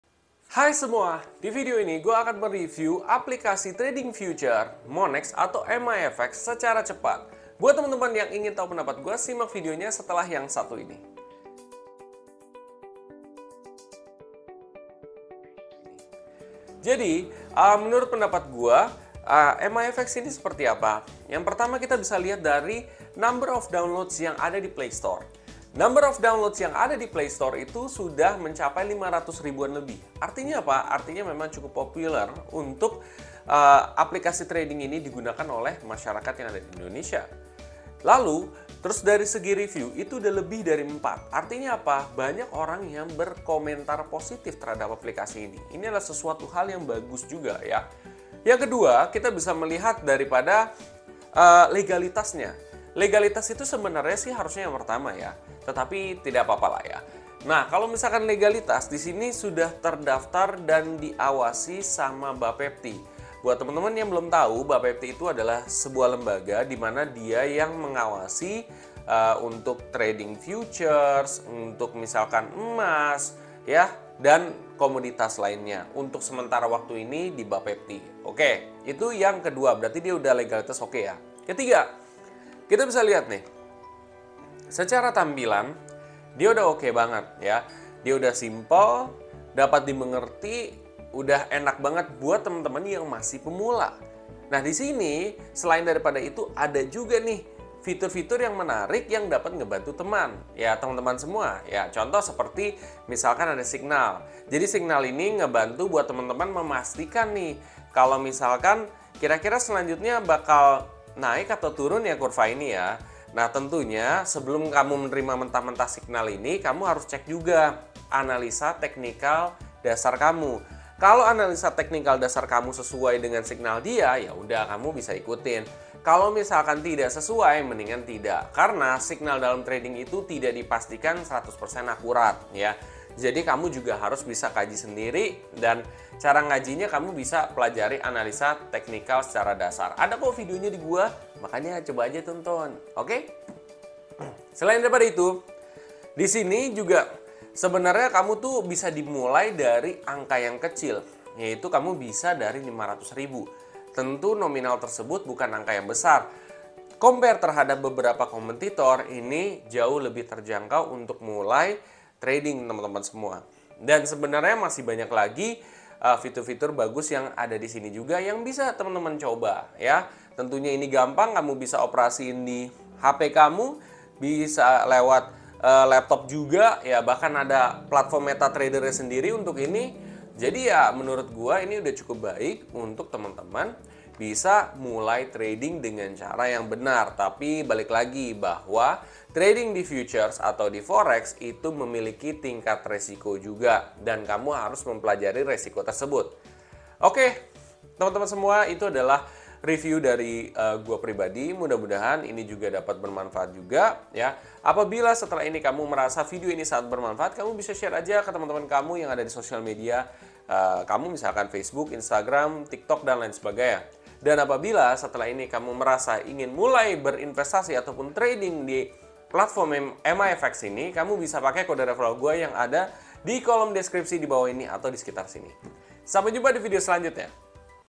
REVIEW APLIKASI TRADING FOREX MIFX DALAM 4 MENIT
REVIEW TRADING FOREX MIFX DALAM 4 MENIT || REVIEW MIFX

Memilih aplikasi trading atau investasi itu harus berhati hati, terutama aplikasi forex. Aplikasi forex yang terdaftar dan diawasi BAPPEBTI itu masih sedikit, salah satunya aplikasi MIFX ini. Buat kamu yang ingin tau pendapat saya full terhadap aplikasi ini, plus dan minusnya, simak video ini secara full.

atau

Buat kamu yang mau bantu channel ini maju bisa donasi ke sini